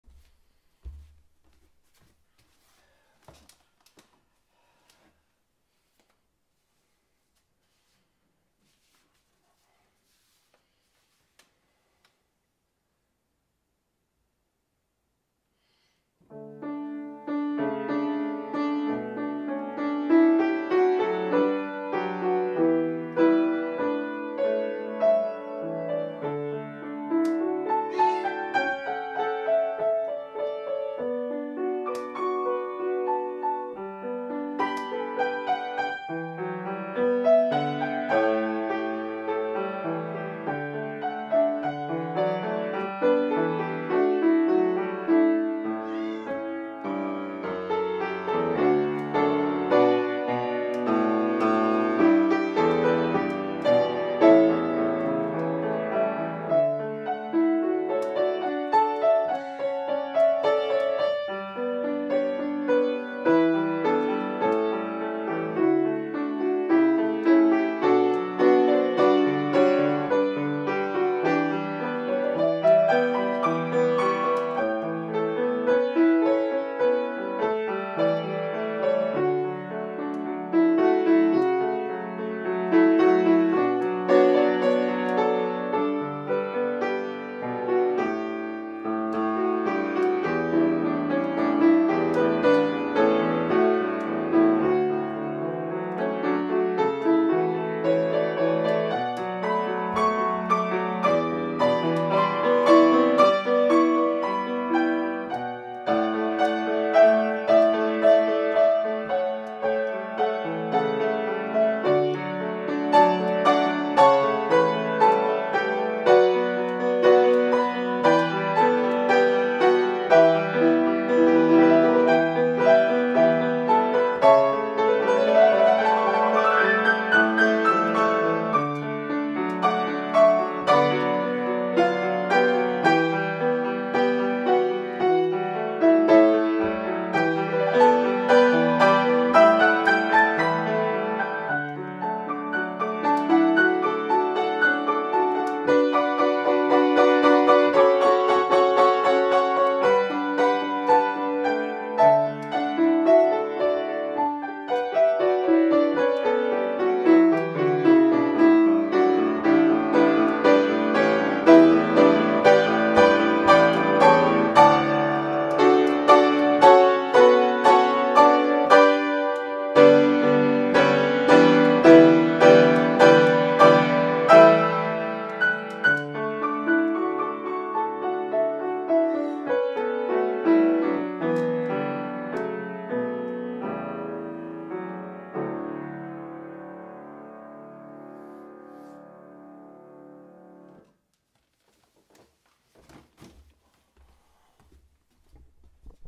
Piano in the dark - improvising when you can't see the keyboard
Improvising at the piano with a sleep mask on.  This is a fun exercise in muscle memory.  I am always fascinated by how much my fingers remember without seeing the keyboard.